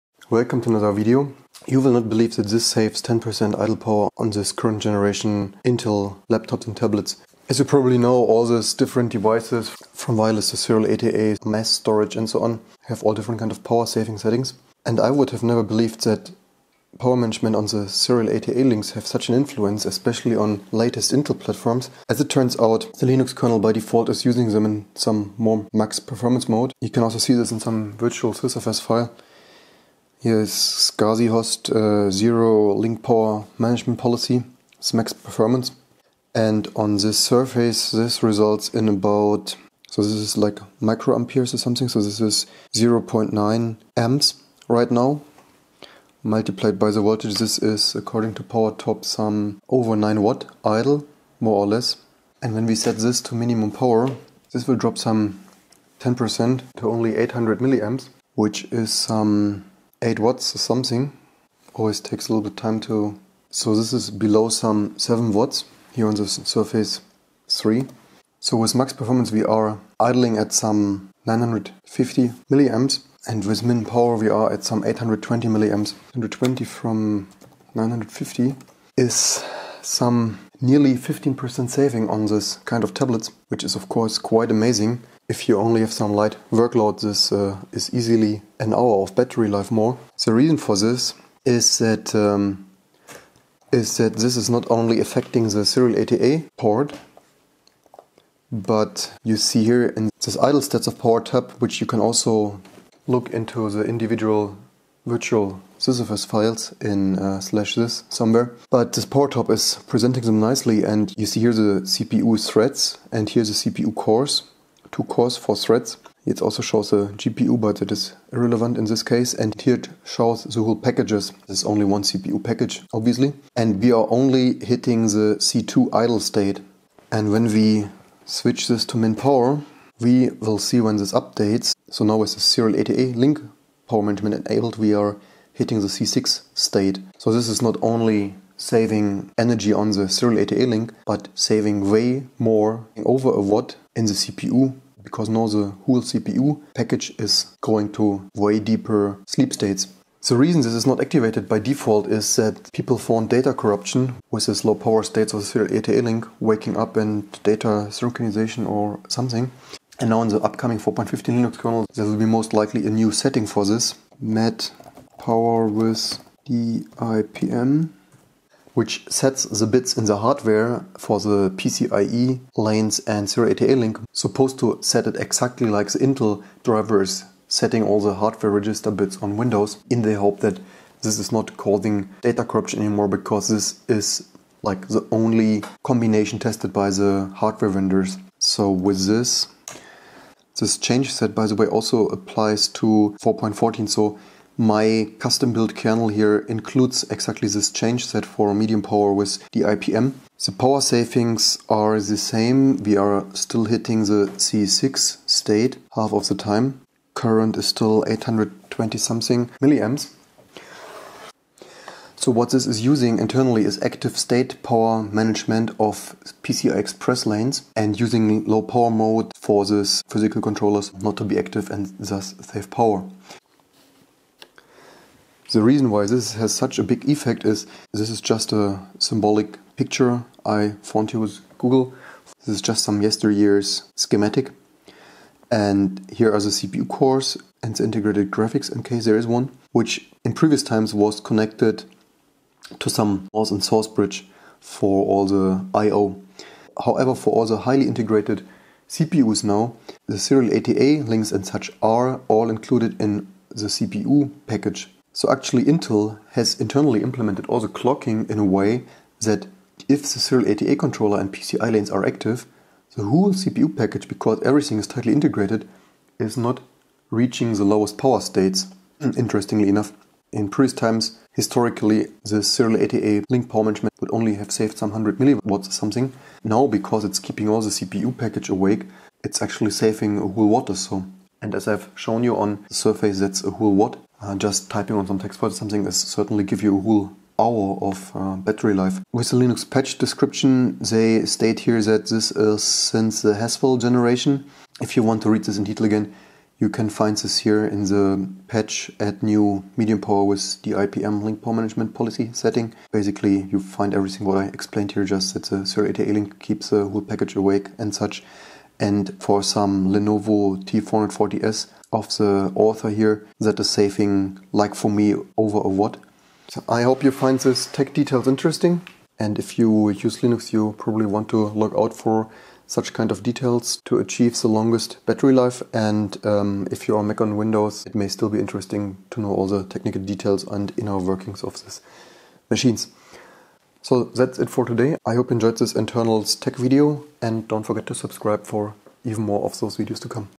Saving 〉1 Watt, 15% idle power! Intel Core-i, Linux PCIe SATA Aggressive Link Power Management
More than 1 Watt power saving with SATA, PCIE, Active State Power Management, more than 15% on a Microsoft Surface Pro 3. Easily gain an hour or more battery run-time!